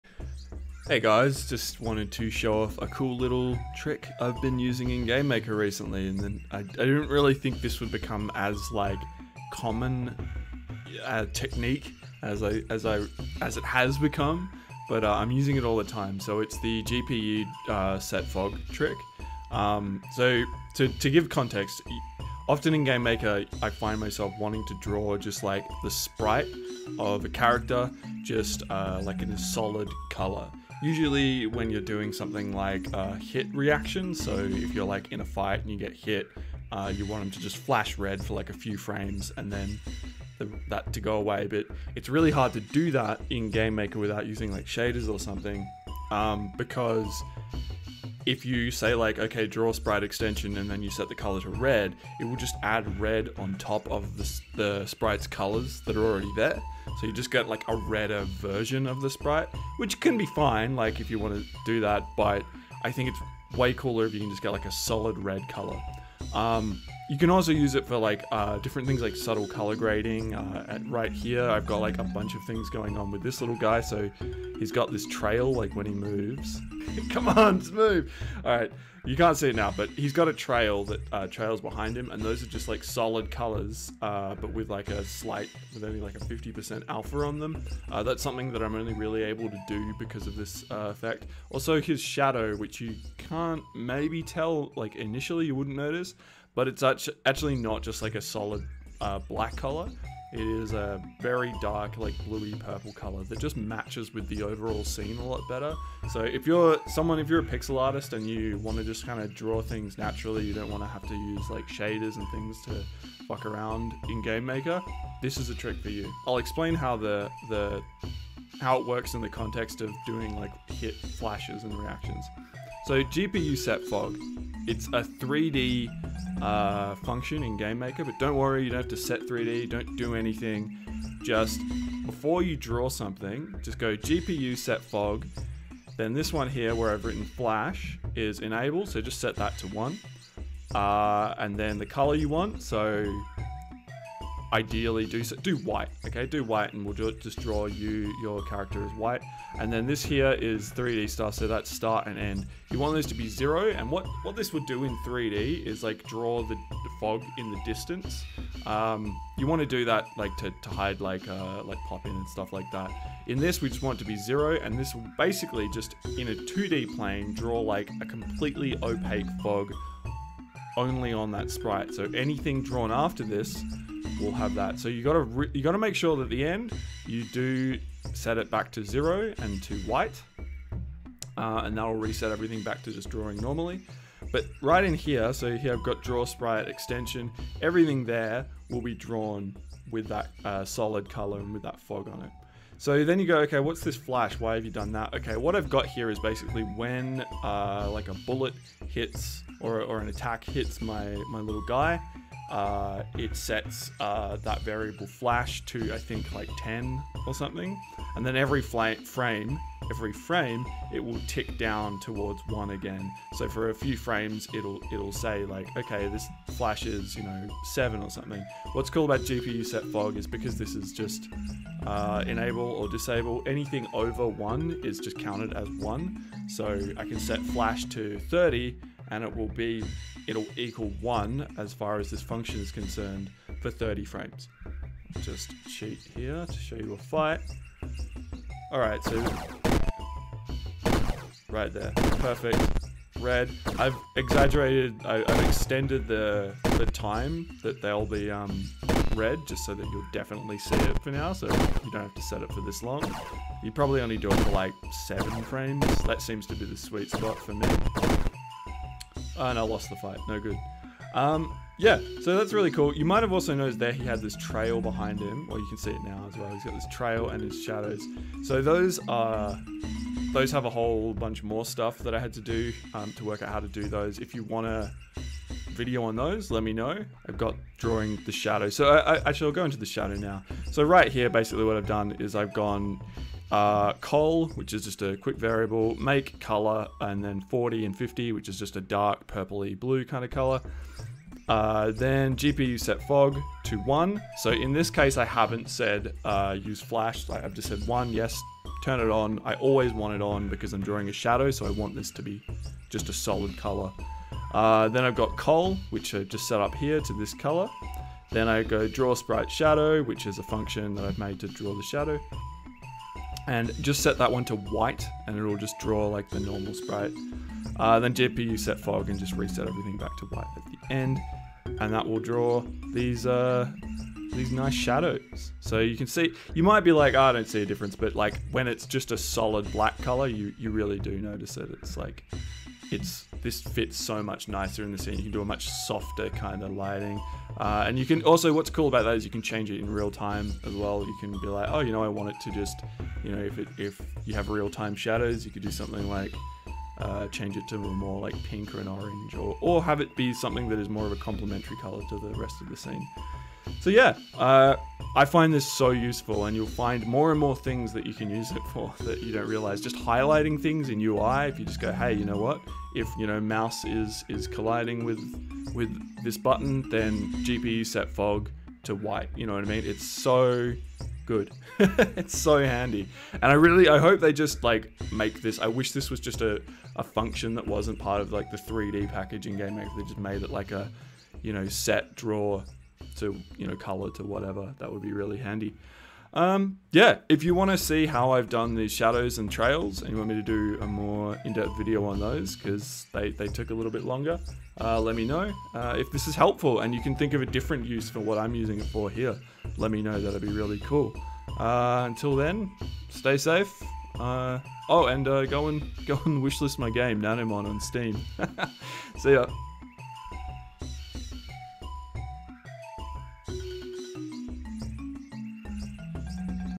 Game Maker Sprite recolor w/o shader // Tutorial
A simple, yet incredibly useful trick in game maker.